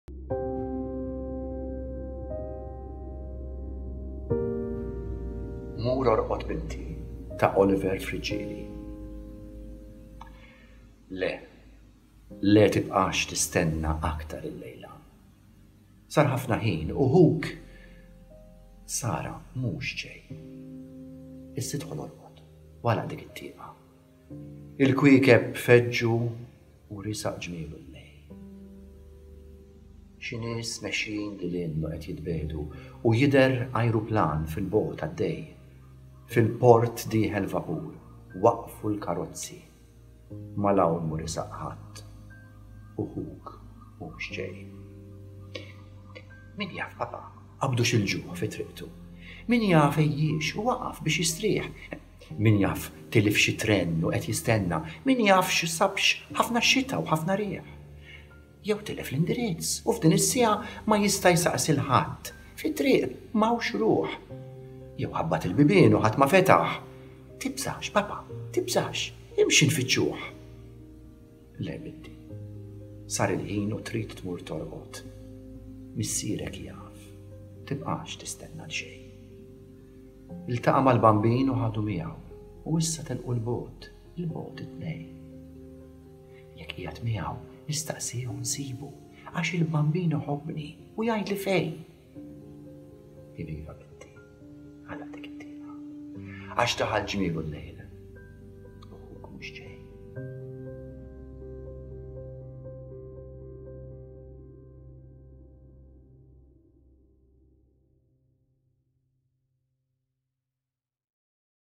Mur Orqod, Binti ta' Oliver Friggieri
Mur Orqod, Binti
(it-tieni eleġija għat-tieni tifel li ġie u telaq)
ta' Oliver Friggieri

“Le, le tibqax tistenna aktar illejla,
Sar ħafna ħin u ħuk, Sara mhux ġej,
Iss’idħol orqod w agħlaq dik it-tieqa,
Il-kwiekeb feġġu w riesaq ġmielu l-lej’.

Xi nies mexjin lil hinn u qed jitbiegħdu,
U jidher ajruplan fil-bogħod għaddej,
Fil-port dieħel vapur, waqfu l-karozzi,
’mma ’l hawn mhu riesaq ħadd u ħuk mhux ġej.”

“Min jaf papà, qabdux il-ġuħ fi triqtu?
Min jaf għejiex u waqaf biex jistrieħ?
Min jaf tilifx it-tren u qed jistenna?
Min jaf sabx ħafna xita w ħafna riħ?

Jew tilef l-indirizz u f’din is-siegħa
Ma jista’ jsaqsi ’l ħadd... fit-triq m’hawnx ruħ.
Jew ħabbat il-bibien u ħadd ma fetaħ.
Tibżax, papà, tibżax, imxi nfittxuh.”

“Le, binti, sar il-ħin u trid tmur torqod.
Missierek jaf; tibqax tistenna għalxej’.
Iltaqa’ mal-Bambin u ħadu miegħu
U issa telqu ’l bogħod ’il bogħod it-tnej'."

“Jekk qiegħed Miegħu nistaqsih u nsibu
Għax il-Bambin iħobbni u jgħidli fej”
“Iv’iva binti, agħlaq dik it-tieqa
Għax daħal ġmielu l-lej’ u ħuk mhux ġej.”

Ħajr lil Ray Calleja u lil Graziella Ellul tal-filmat.